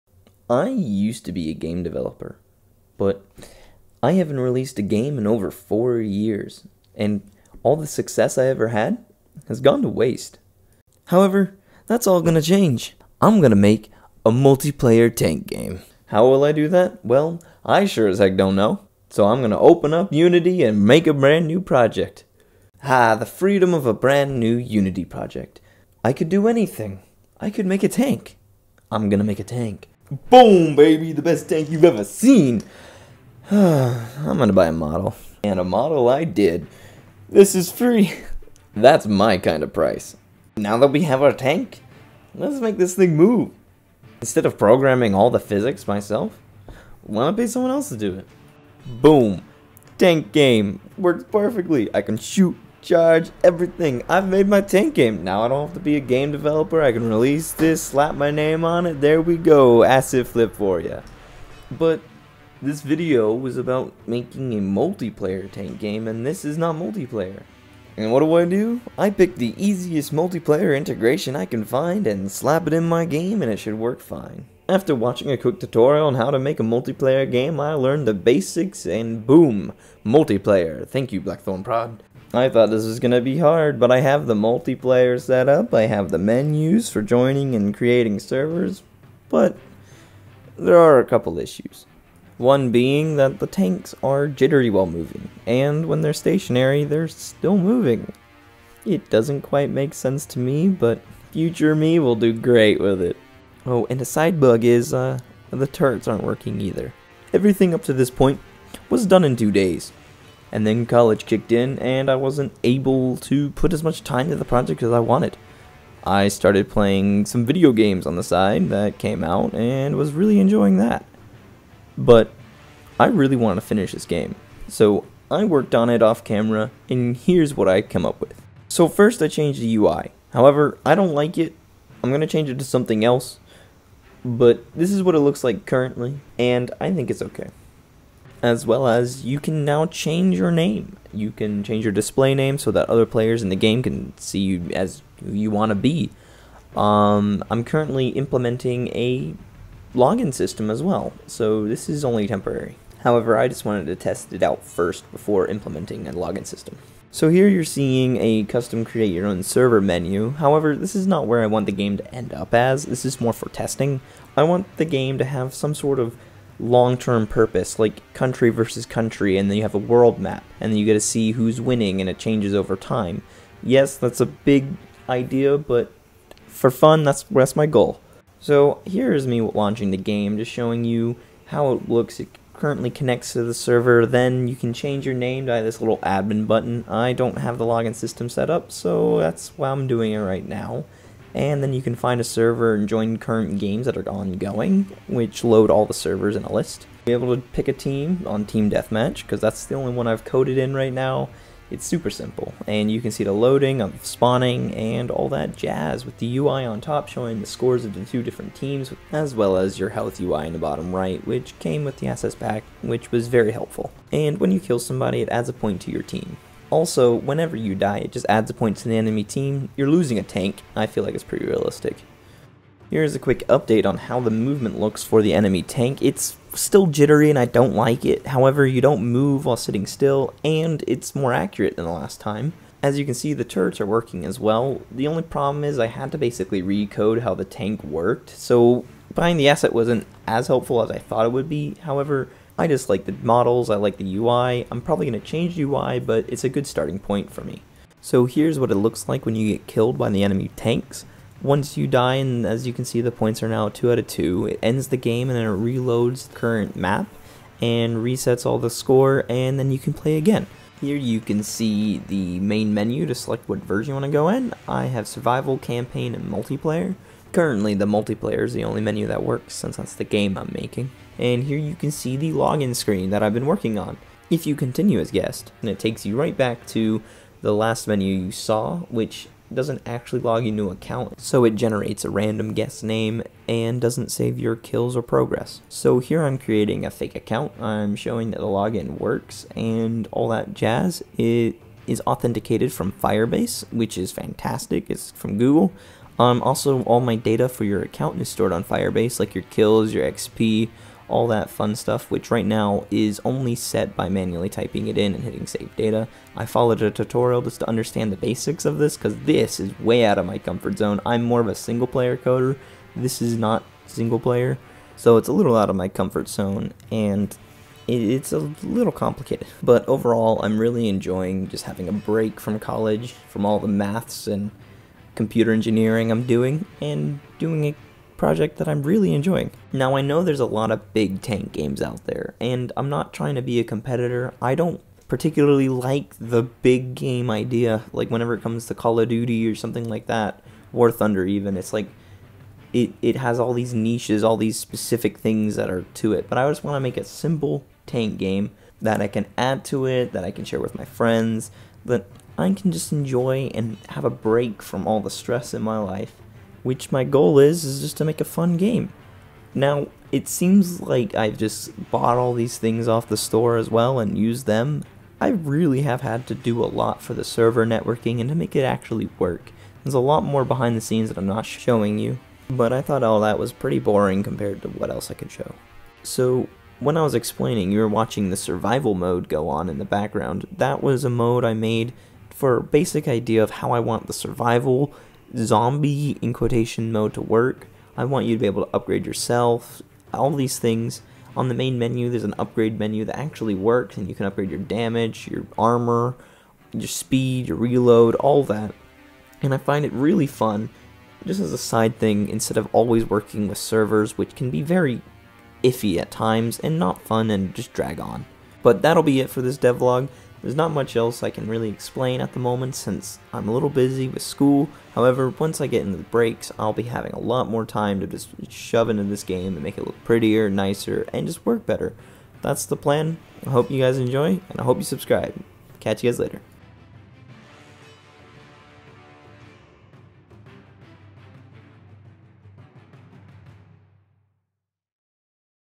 Creating a multiplayer tank game! Unity Devlog 1
MBTanks is an online tank war game where your goal is to conquer the world with your faction while upgrading your personal loadout with your dream tank setup.

I haven't made a youtube video in over two years. This was definitely a new experience!